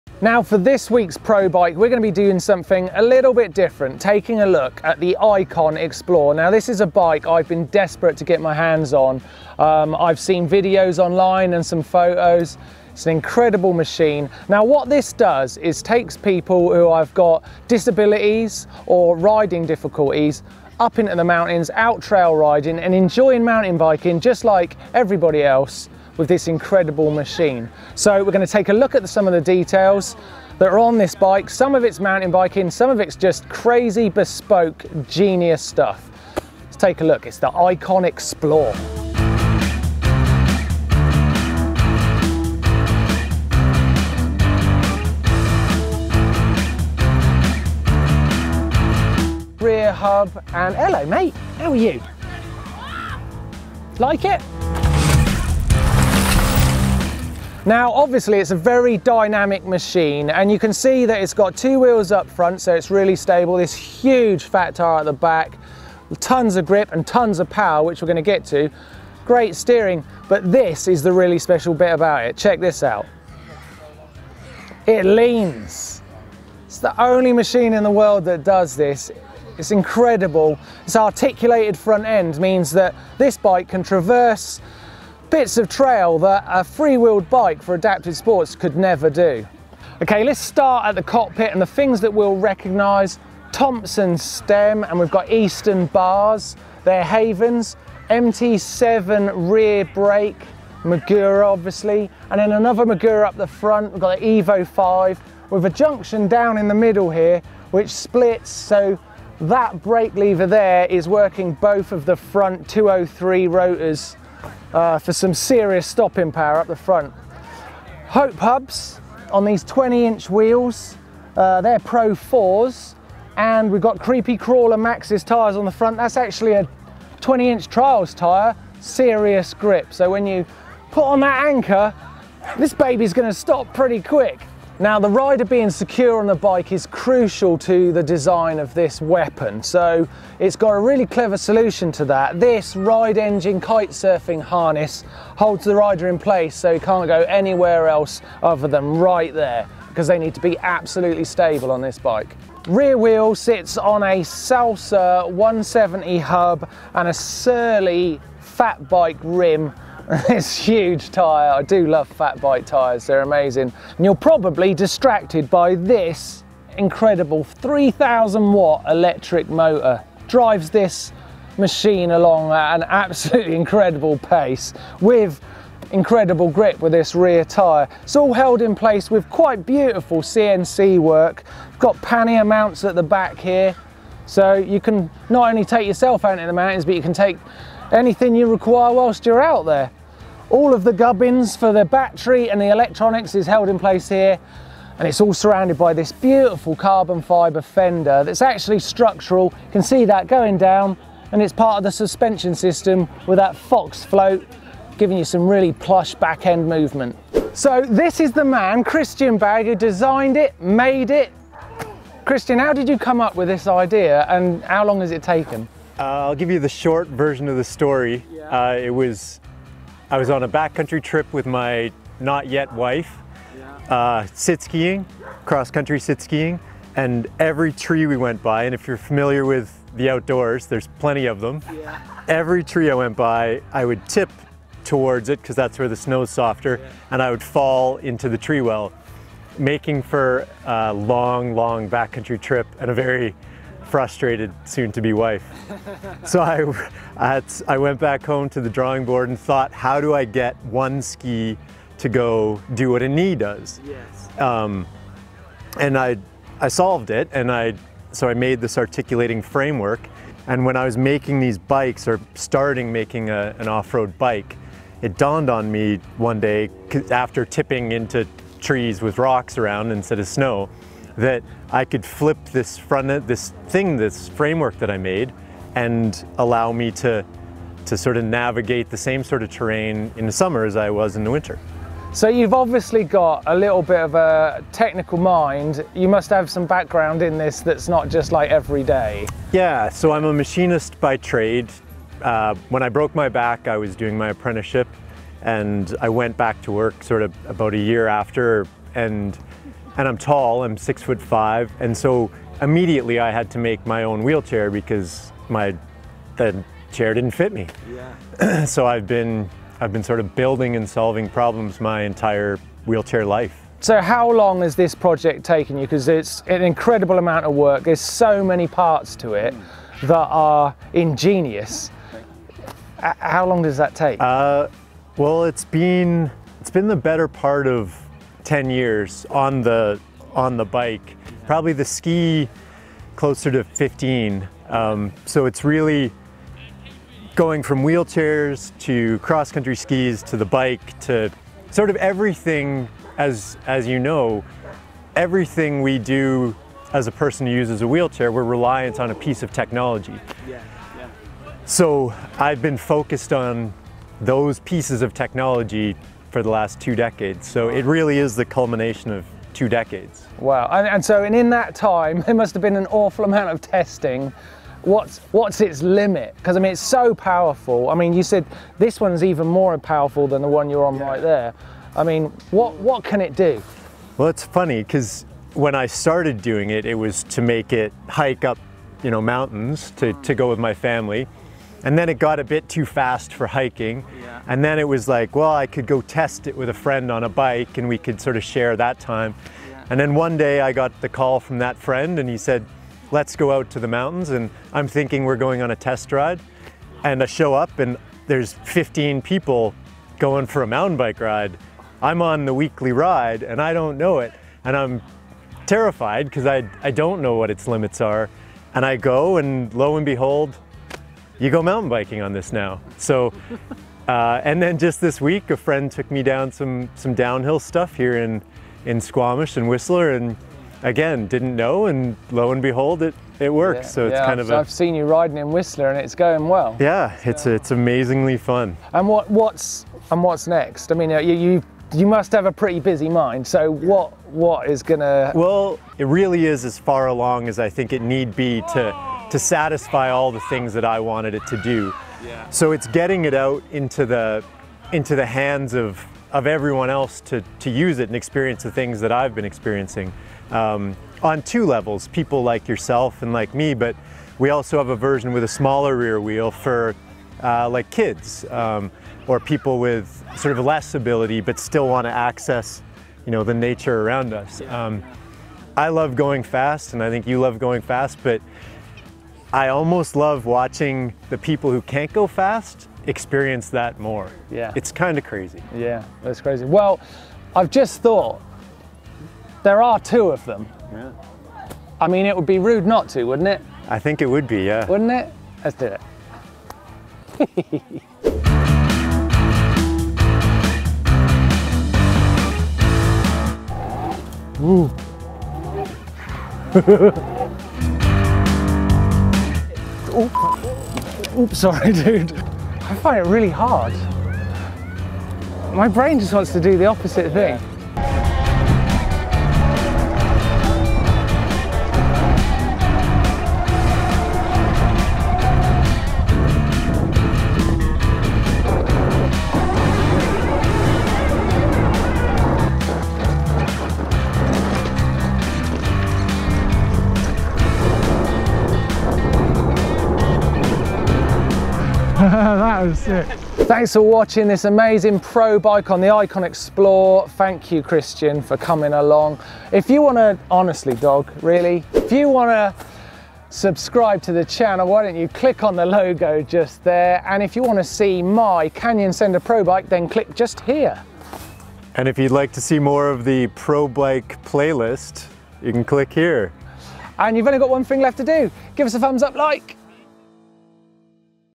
The Icon Explore Articulated Mountain Trike | GMBN Pro Bikes
We've got something a little different for todays Pro Bike! Icon specialise in making custom wheelchairs and all kinds of machines to help disabled people who love the great outdoors explore the world around them.

Christian Bagg, Icon's founder, has single handedly developed the incredible articulated system featured on this trike, the Icon Explore. This allows the two front wheels to move independently of each other, meaning this electric powered mountain trike can lean with terrain and explore the mountains with ease!

Frame: 316 Stainless Steel Aerospace Grade Icon Explore frame
Shock: A Fox Float for every wheel!
Wheels: 20" trials wheels on Hope Pro4 hubs up front with a Surly Fatbike wheel w/ Salsa 170mm hub at the rear
Tyres: Maxxis 20" Creepy Crawler Trials tyres up front with a Schwalbe Jumbo Jim fatbike tyre on the rear
Bars: Easton Haven
Grips: Twist to go Cyclone motorbike grip
Motor: 3000w Cyclone Mid-Drive electric Motor
Stem: Thomson 80mm mountain bike stem
Seat: Custom sized carbon fibre seat pan with a RideEngine Kitesurfing harness for security
Brakes: Magura MT7 Brakes
Rotors: 203mm Magura rotors all round

Extra bits: The incredible Icon Carver articulated system allows the front wheels to lean with the terrain! The carbon fibre rear fender is integrated into the frame and rear suspension linkage!

If you'd like to contribute captions and video info in your language, here's the link 👍